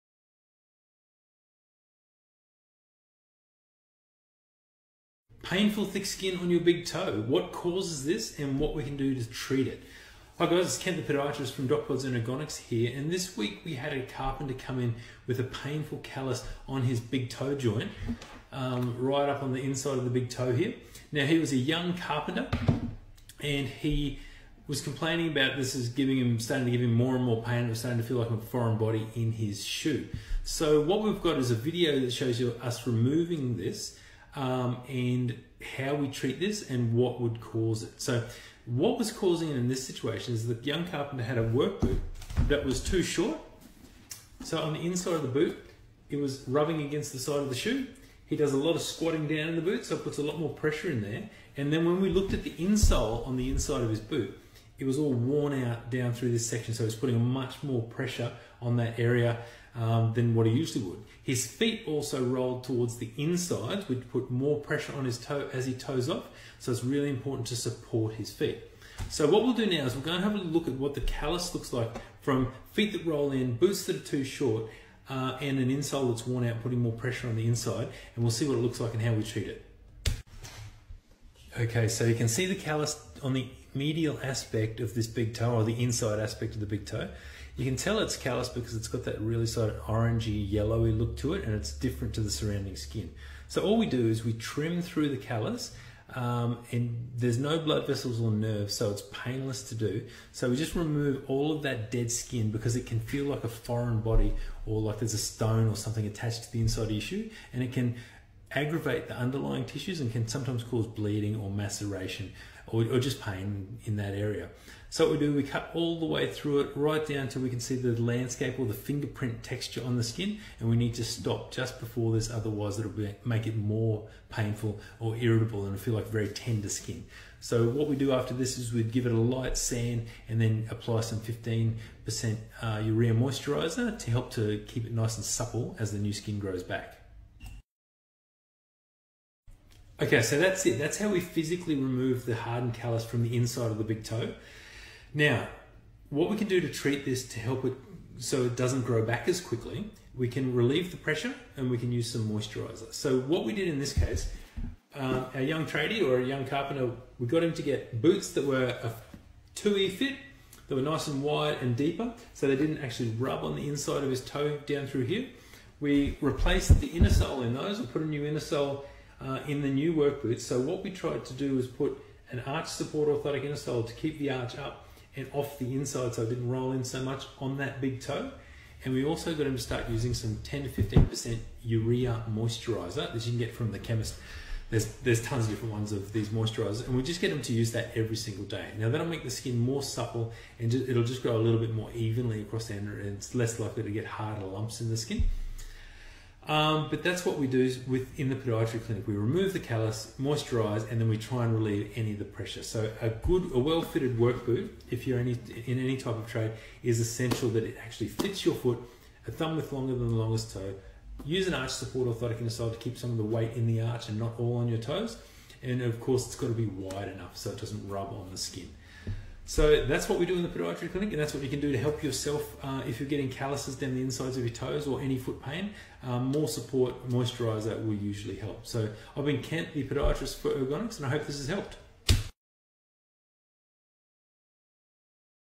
Painful thick skin (callus) on the big toe of a Carpenter_- What can we do to fix it callus corn #
Painful thick skin (callus) on the big toe of a Carpenter_- What can we do to fix it